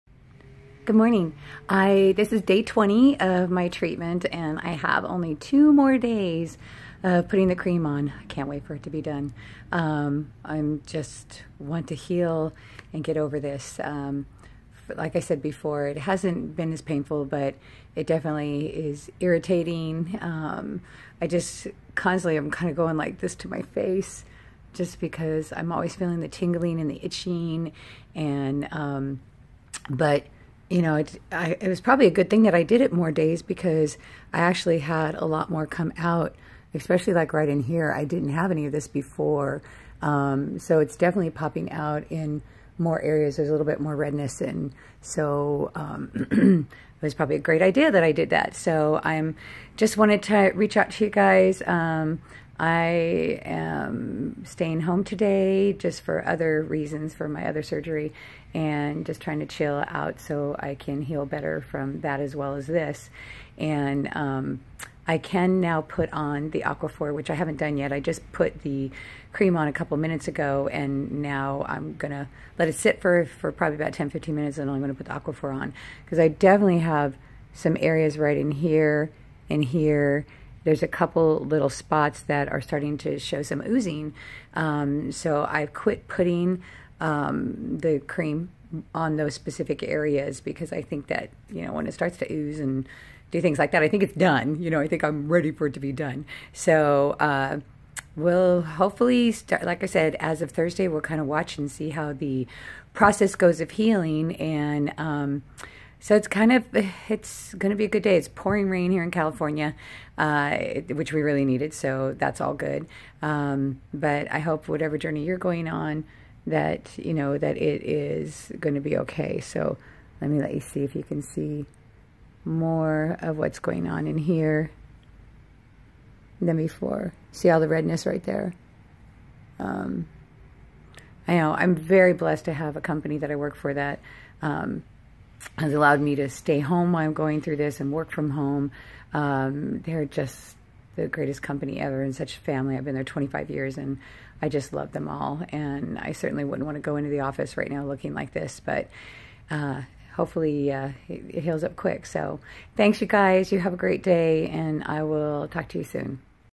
Skin cancer Fluorouracil Topical treatment Day 20
Carac is the trade name for fluorouracil cream. 5-FU cream and efudex and fluoroplex are other names for fluorouracil cream.  Fluorouracil is an anti-cancer chemotherapy drug.